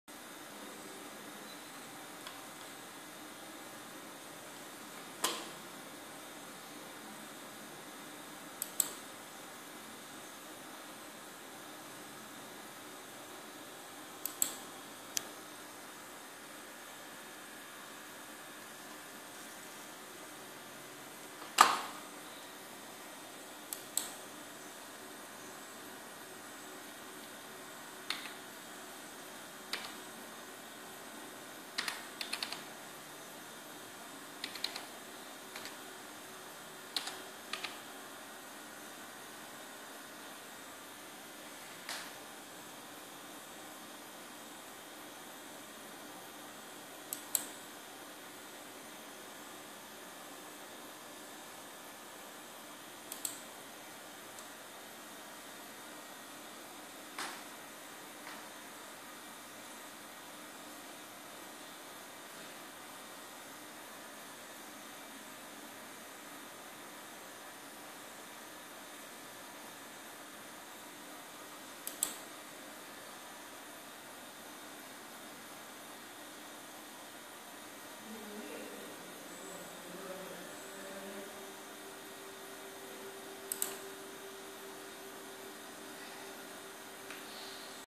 Add or Remove Programs on Windows 10
Windows 10  Control Panel to add or remove a desktop program.
Uninstall Programs can also done by Start menu or the Settings app instead.